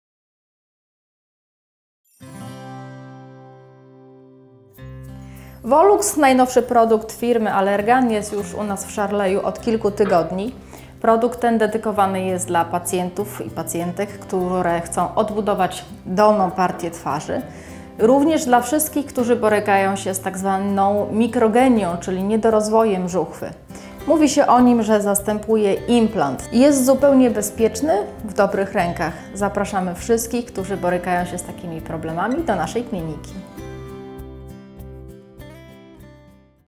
Wolumetria twarzy - Juvederm Volux
Chcesz wymodelować swoją brodę i wyglądać pięknie? Zapraszamy na zabiegi wolumetrii twarzy do Kliniki Sharley!

Zapraszamy na Zabiegi!
Sharley Medical Clinic & Day SPA
Warszawa, Al. Jana Pawła II 75